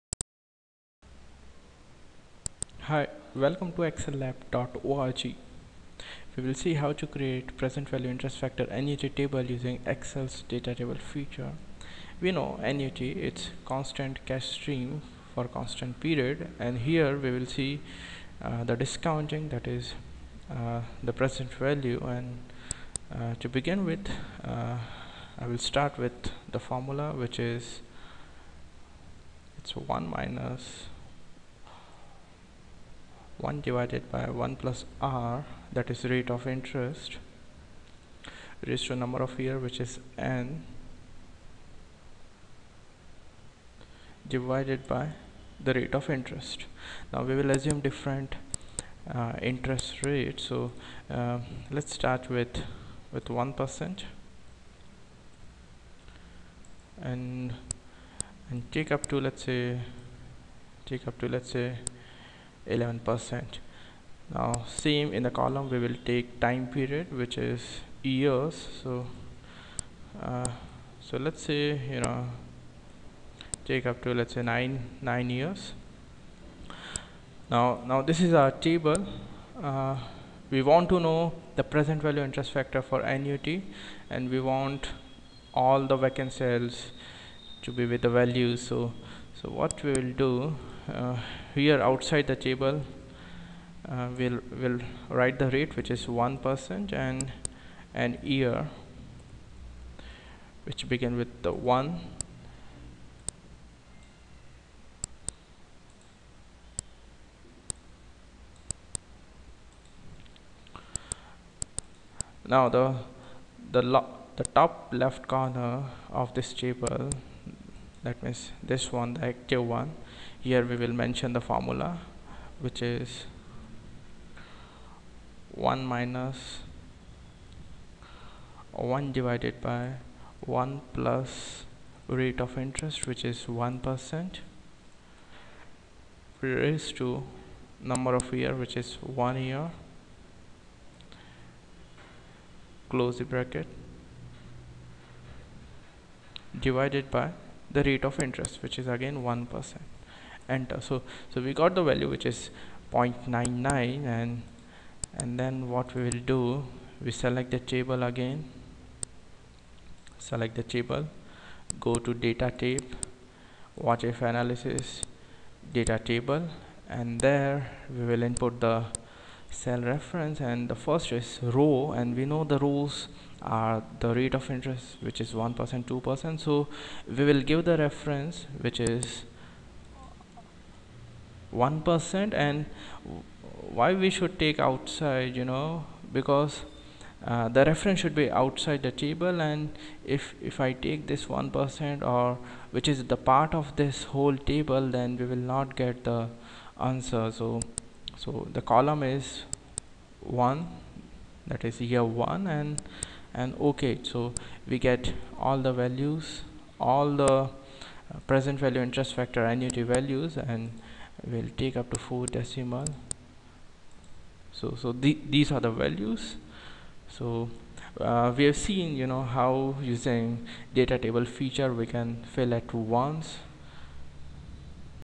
MS Excel data table Time Value of Money - Pesent value Interest factor Annuity table
Creating Pesent value Interest factor Annuity table using Excel's data table